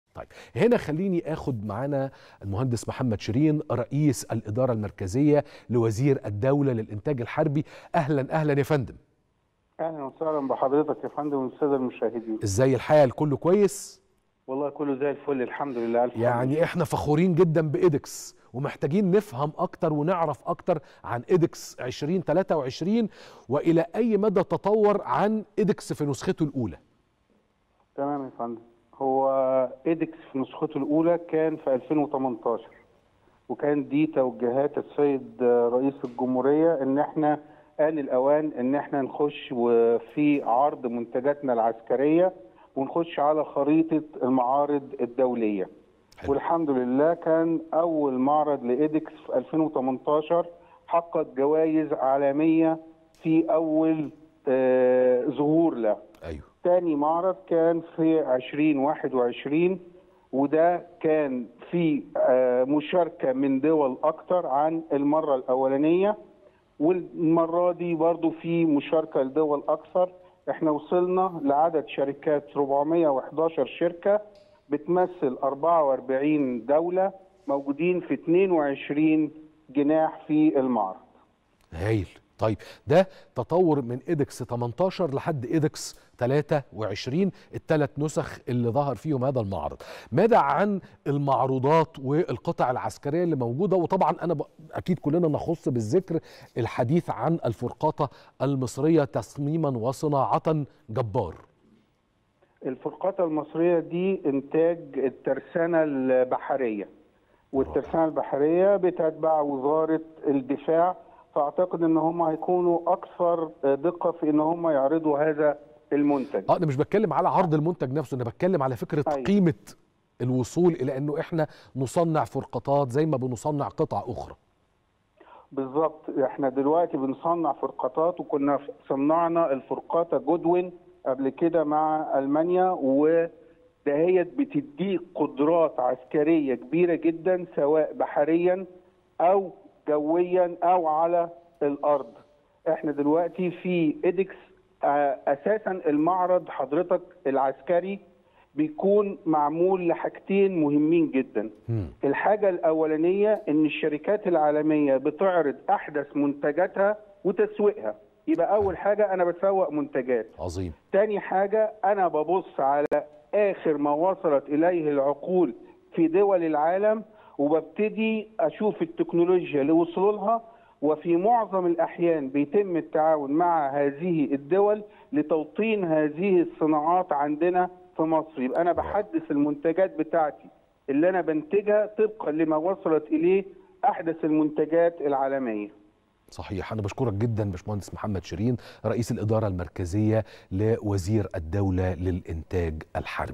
رئيس الإدارة المركزية لوزير الدولة للإنتاج الحربي يوضح حجم التطور لـمعرض إيدكس2023 عن النسخ السابقة
رئيس الإدارة المركزية لوزير الدولة للإنتاج الحربي يوضح حجم التطور لـ معرض إيدكس 2023 عن النسخ السابقة